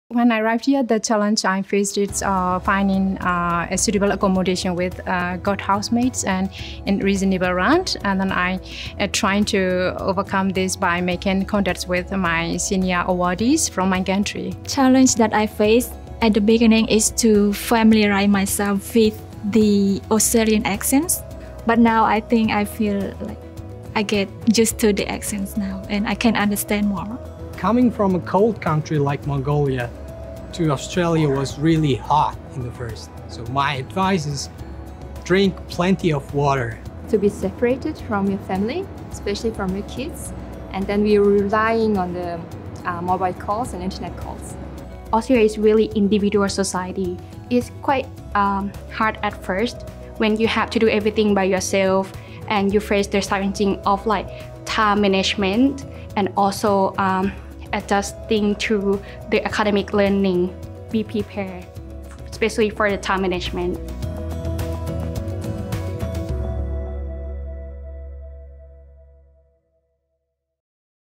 Challenges to overcome - advice from Mekong & Mongolian scholars
Australia Awards scholarship recipients from the Mekong and Mongolia studying in Australia answer the question: What was the biggest challenge you faced arriving in Australia and what steps should scholars take to be prepared?